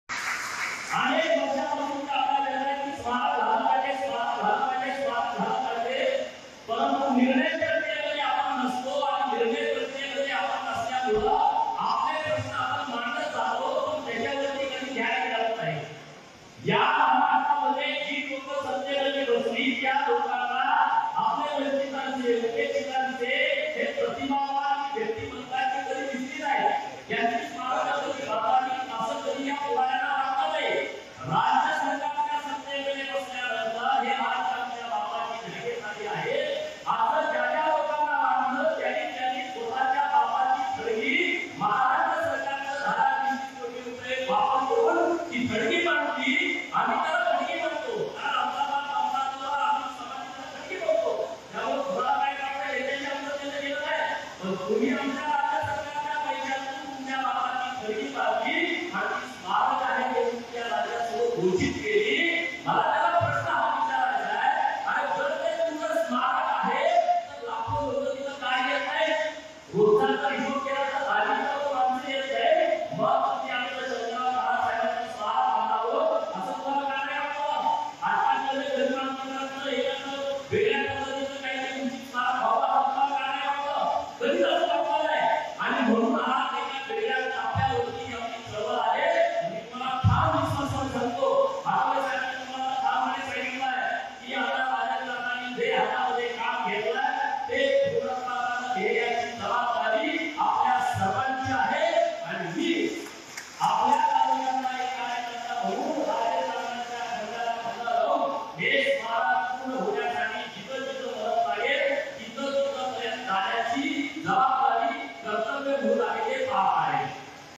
सरकार बापाची जहागिरदारी वाटली; Gopichand Padalkar यांचा ठाकरे सरकारवर घणाघात | Maharashtra Times
आमदार गोपीचंद पडळकर यांनी महाविकास आघाडी सरकारवर घणाघात केली आहे.
महाराष्ट्र हा आपल्या बापाची जहागिरदारी समजून सत्ताधाऱ्यांनी आपल्या बापांची स्मारके जाहीर केली असं ते म्हणाले.
सरकारी तिजोरीतून आपल्या बापांची  थडगी बांधली,असा घणाघात भाजपाचे आमदार गोपीचंद पडळकर यांनी केला आहे.
महाराष्ट्रातील उपेक्षित आणि प्रतिभावंत व्यक्ती यांची स्मारके बांधावी असं का वाटलं नाही असा सवाल त्यांनी केला.